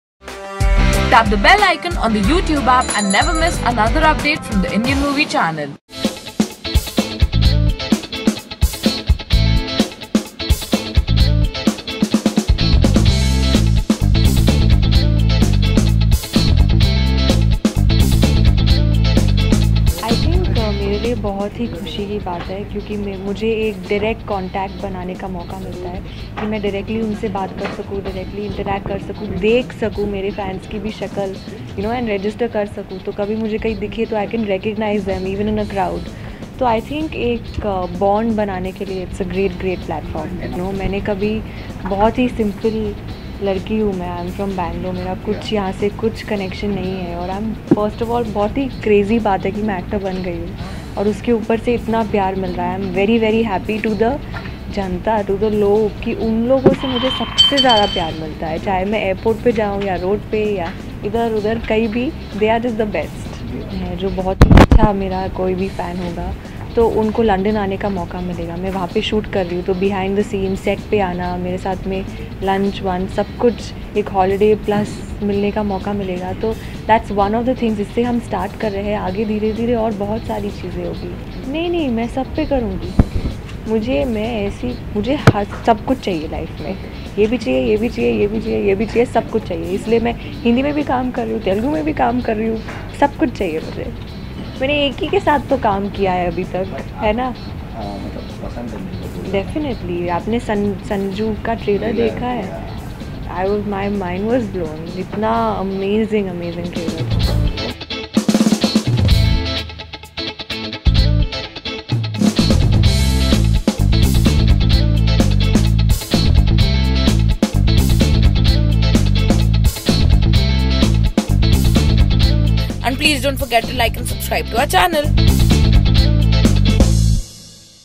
Nidhhi Agerwal launches her own application
Actress Nidhhi Agerwal who made her Bollywood debut with Tiger Shroff in the movie Munna Michael is all set to get closer to her fans! Munna Michael fame announces the launch of her own mobile application . The application is called “Nidhhi Agerwal Official” which will be available on IOS and Android platforms.